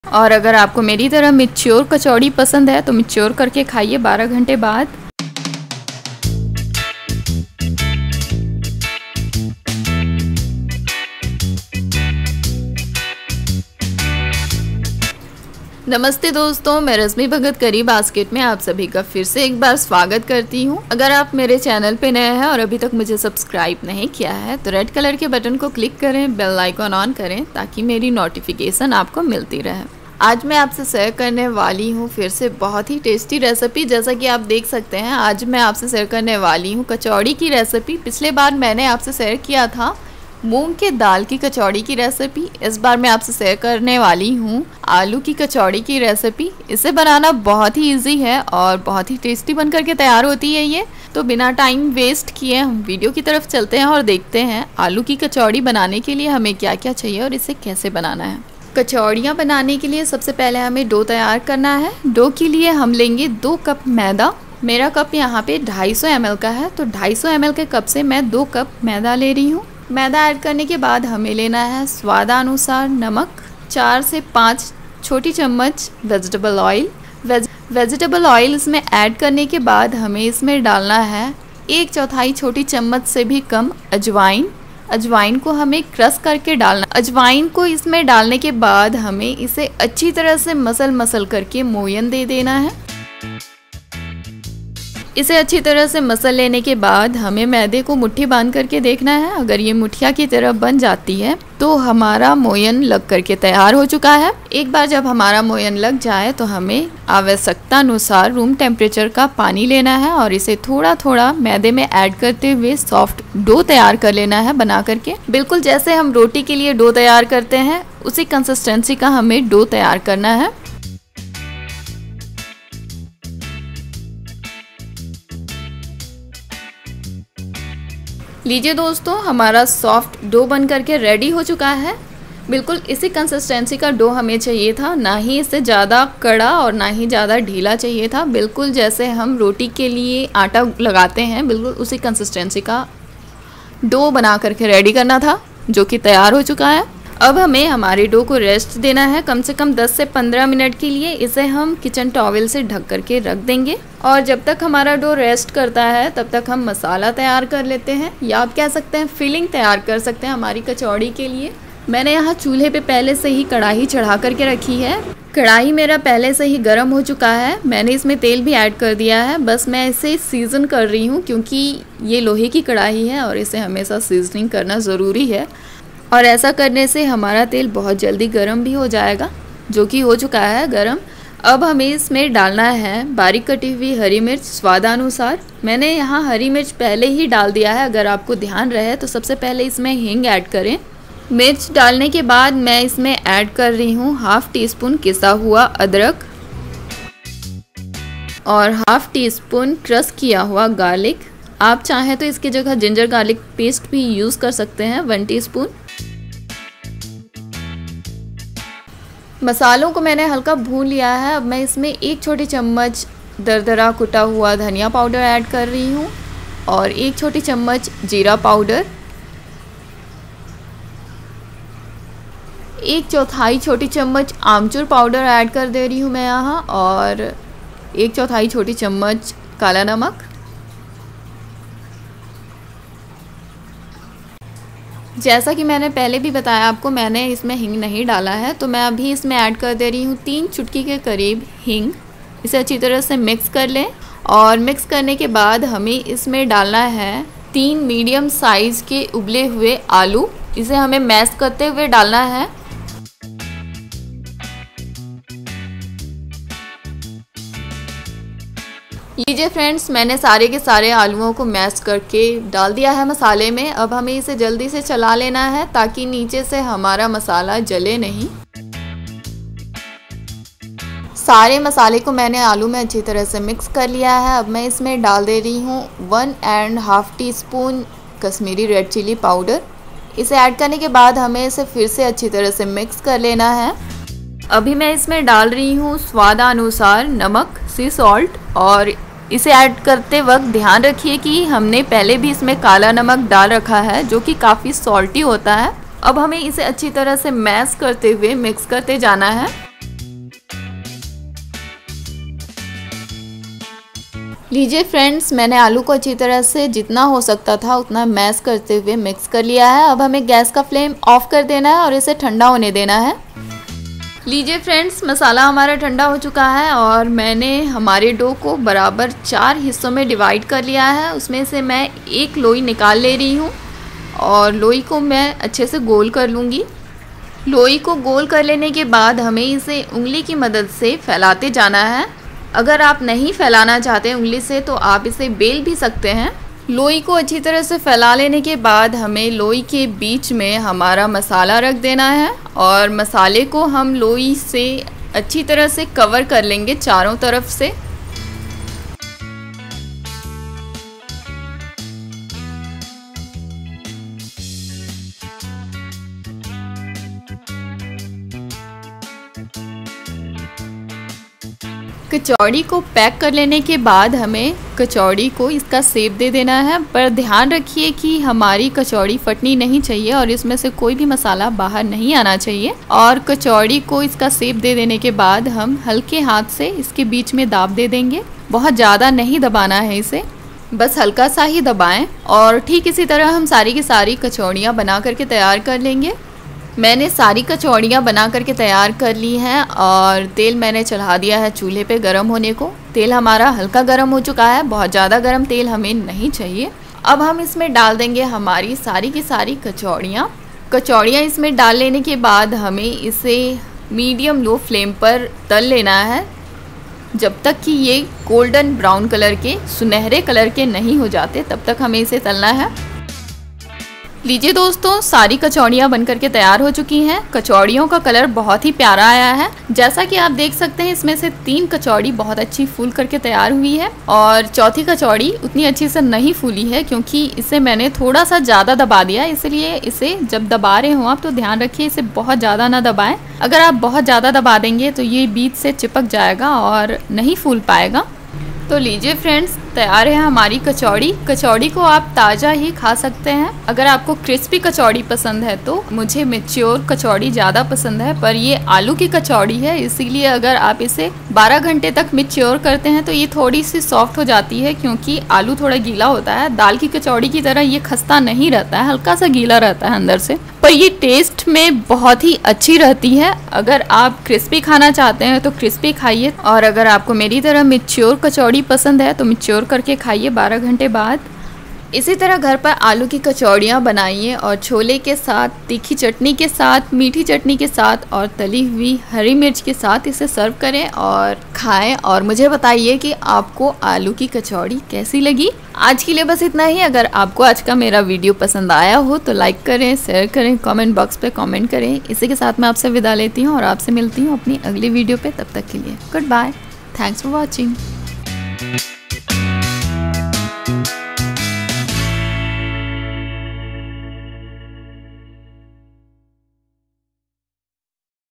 फूली फूली आलू की कचौरी बनाने आसान तरीका हलवाई जैसी कचौरी Kachori recipe
Hii i am Rashmi Bhagat
and you are watching CURRY BASKET.This is vegan and vegetarian food recipes channel. and this recipe is vegan.

पिछली बार मैंने आपसे मूंग दाल की कचौरी की रेसिपी
शेअर की थी। इस बार में आपसे आलू की कचौरी की रेसिपी शेयर कर रही हूँ।

इसे बनाने के लिए आपको चाहिए

मैदा २ कप
नमक स्वादानुसार
वेजिटेबल आयल ४-५ चम्मच
अजवाइन १/४ चम्मच से कम
पानी आता लगाने के लिए

तेल ४-५ चम्मच
हींग ३ पिंच
बारीक़ कटी हरी मिर्च स्वादानुसार
किसा हुआ अदरक १/२  छोटी चम्मच
किसा हुआ लहसुन १/२ छोटी चम्मच
दरदरा कुटा हुआ धनिया १ छोटी चम्मच
जीरा पाउडर १ छोटी चम्मच
आमचूर पाउडर १/४ छोटी चम्मच
कला नमक १/४ छोटी चम्मच
उबला हुआ आलू ३
कश्मीरी लाल मिर्च पाउडर १ १/२ टीएसपी
नमक स्वादानुसार

विधि

कचोरी के लिए हमे आता लगाना है. मैदे में नमक ४ - ५ चम्मच तेल और अजवाइन को क्रस कर के डालें। अब मैदे में थोड़ा थोड़ा पानी डालते हुए सॉफ्ट दोघ तैयार करे बिलकुल रोटी के आटे के जैसे और इसे धक् कर १५ मिनट का आराम दें।

एक कड़ाही चूल्हे पे गरम करें ४-५ चम्मच तेल डालें और तेल गर्म होने दें। तेल के गर्म होने पे इसमें हींग डालें। हरी मिर्च, अदरक और लहसन डालें। अब इसमें सरे मसाले और आलू दाल दें, नमक डालें, और इसे अच्छी तरह मसलते हुए २ मिनट तक भुने।

अब १ लोई का टुकड़ा लें और लोई में मसाले भर कर कचौरी का आकर दें। और कचोरी को हलके गर्म तेल में मध्यम से धीमे आंच पे सुनहरे होने तक तलें। और लीजिये दोस्तों तैयार हो गई हमारी कचौरी।